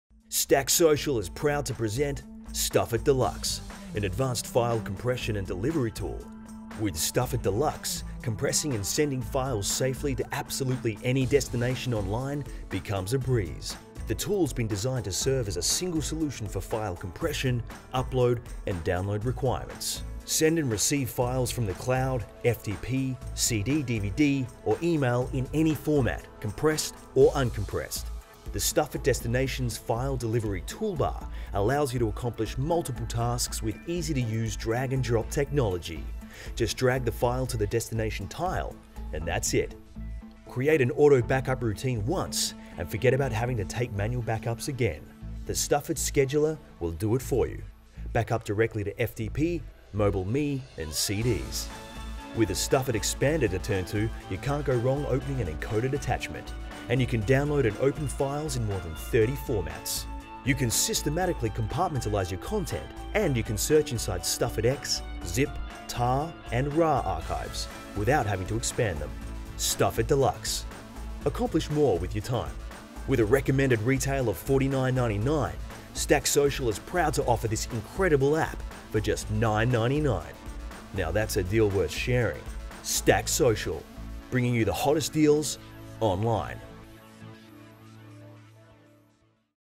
Stuffit Deluxe Mac App from StackSocial
StuffIt Archive Manager provides more advanced features for creating, searching, and browsing archives. And it rocks! Browse & search inside the contents of StuffIt X, Zip, Tar, & RAR archives.

Use StuffIt Scheduler to create an automatic backup routine, one that will sift through your files & back up only those items that have recently changed.

Also includes StuffIt Expander which will open compressed and encoded attachments and file downloads in over thirty different formats including StuffIt, Zip, RAR & Tar.